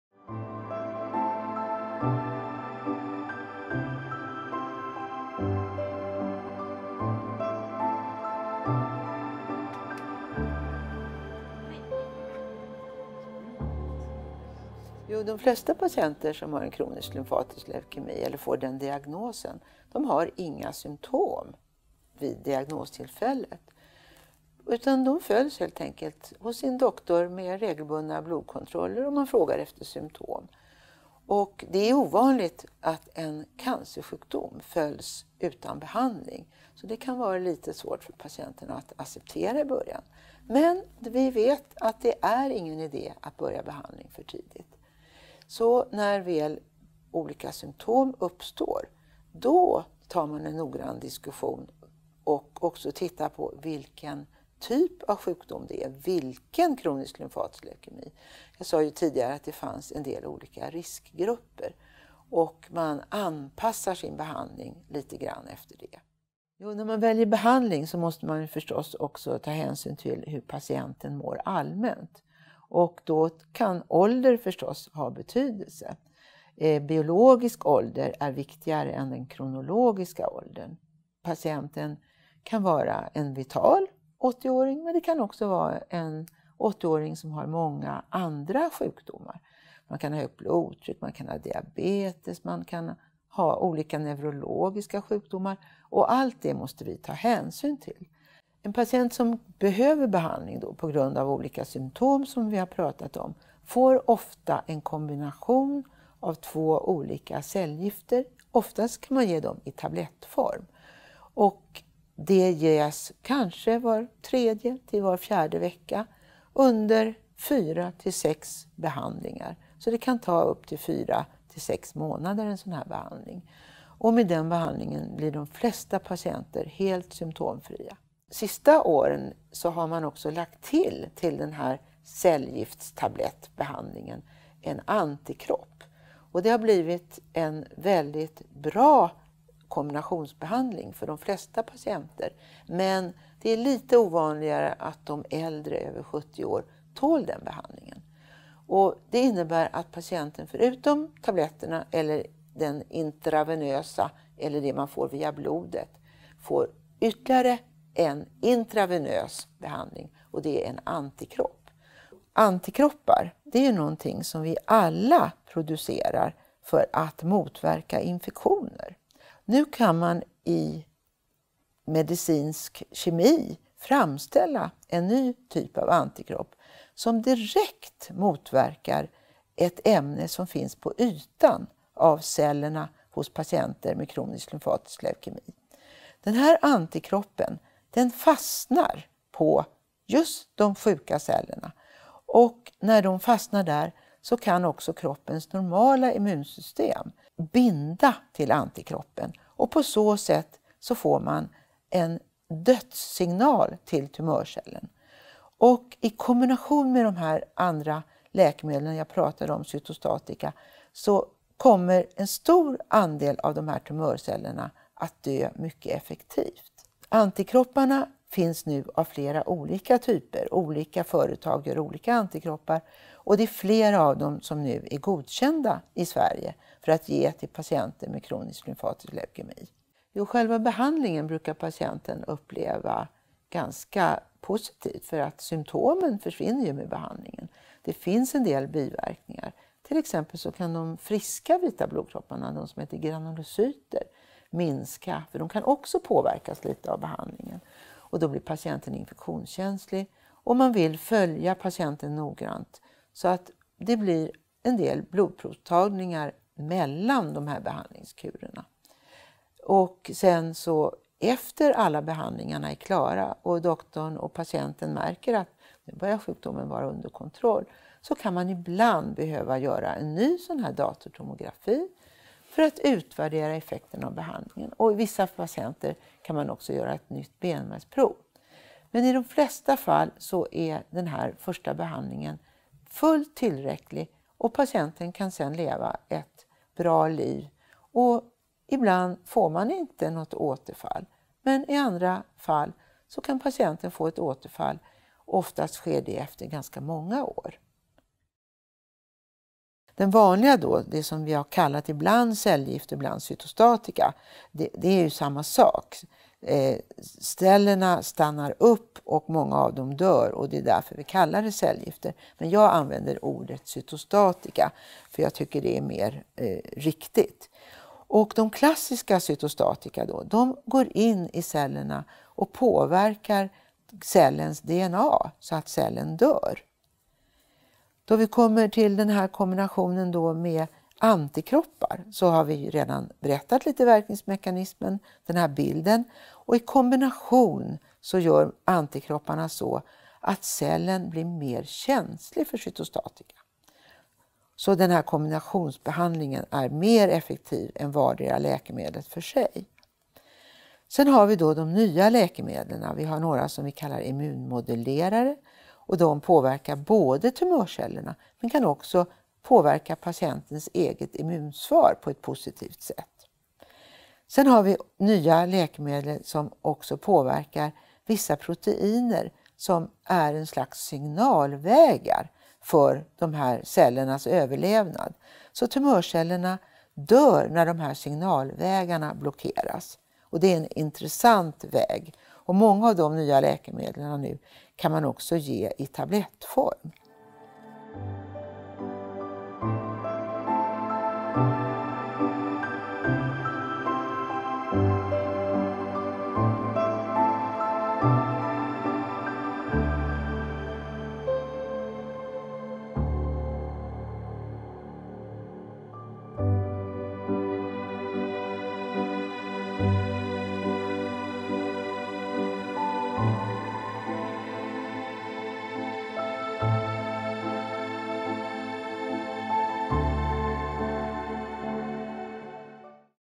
Kronisk lymfatisk leukemi - 4. Behandling
Hur man behandlar den kroniska blodcancerdiagnosen Kronisk lymfatisk leukemi (KLL) förklaras på ett ingående sätt av KLL-experten Eva Kimby.